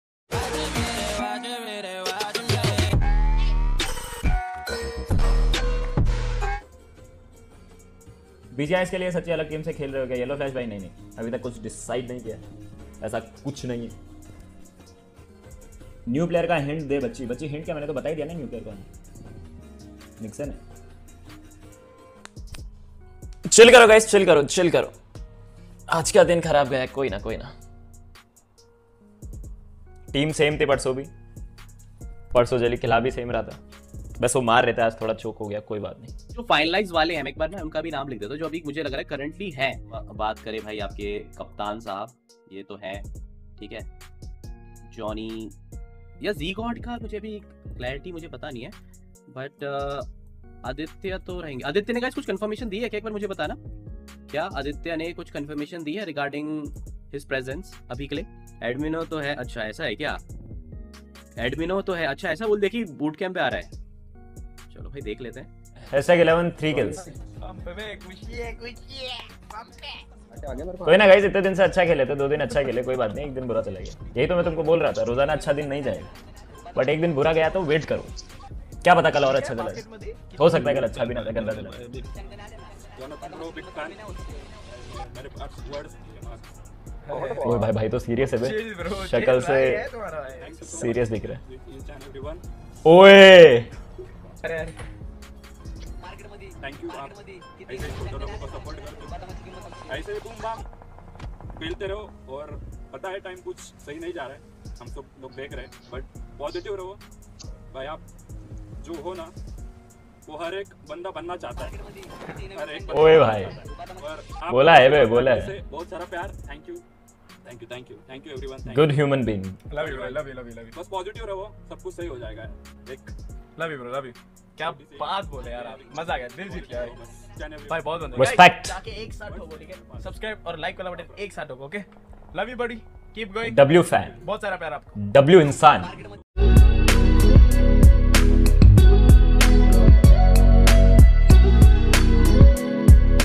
ZGod Reply On GodLike Today's Low Performance😳 GodL Roster After New Addition😲
Credit :- ZGod